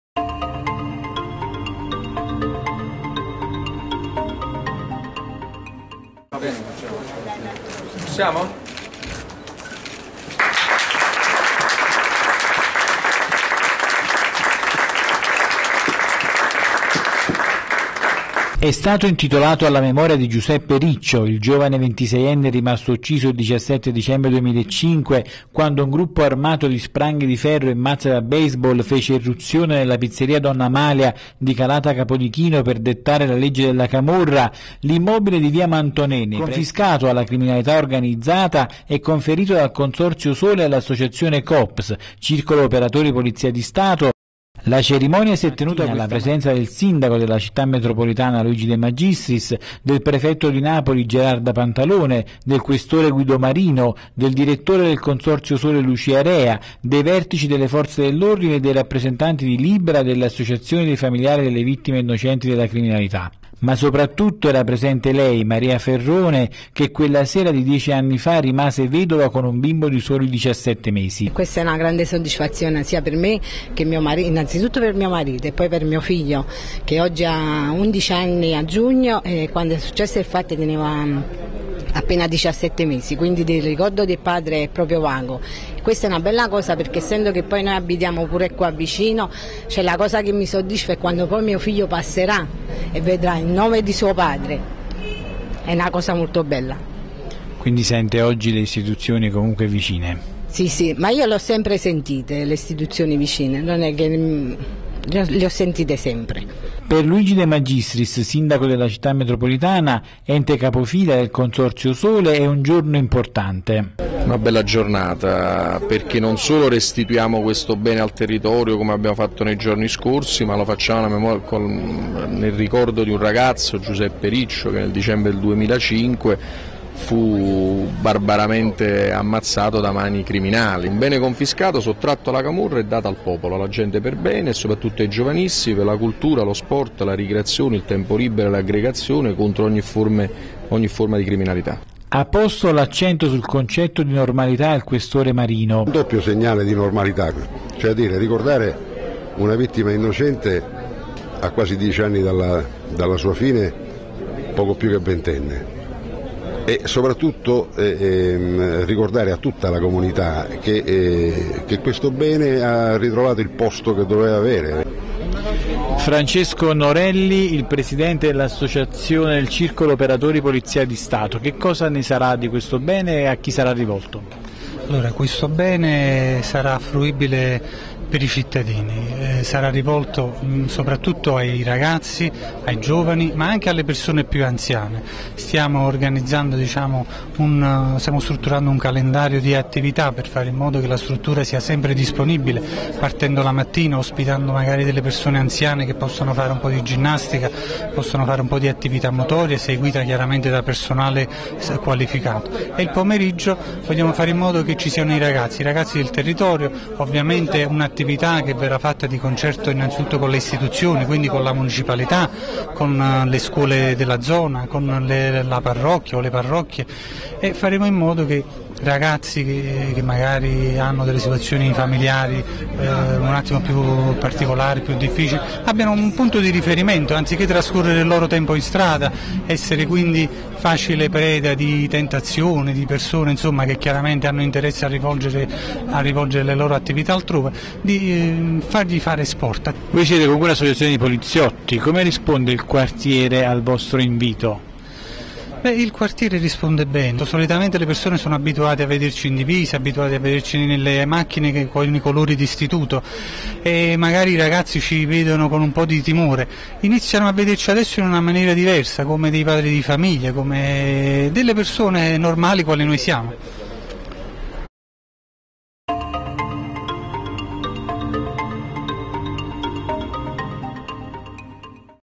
Bene confiscato di via Manthonè intitolato a Giuseppe Riccio, vittima innocente della camorra
L’immobile, che si trova nei pressi di piazza Carlo III a Napoli, è stato ristrutturato dal Consorzio S.O.L.E. che lo ha poi conferito all’Associazione COPS - Circolo Operatori Polizia di Stato – per farlo diventare una palestra per i minori disagiati del quartiere. Presenti la vedova, Maria Ferrone, il Sindaco della Città Metropolitana, Luigi de Magistris, il Prefetto di Napoli Gerarda Pantalone, il Questore Guido Marino, i vertici delle Forze dell’Ordine ed i rappresentanti di Libera e delle associazioni dei familiari delle vittime innocenti della criminalità organizzata.
Il giovane ventiseienne era rimasto ucciso il 17 dicembre 2005, quando un gruppo armato di spranghe di ferro e mazze da baseball fece irruzione nella pizzeria “Donna Amalia” di Calata Capodichino per dettare la legge della camorra.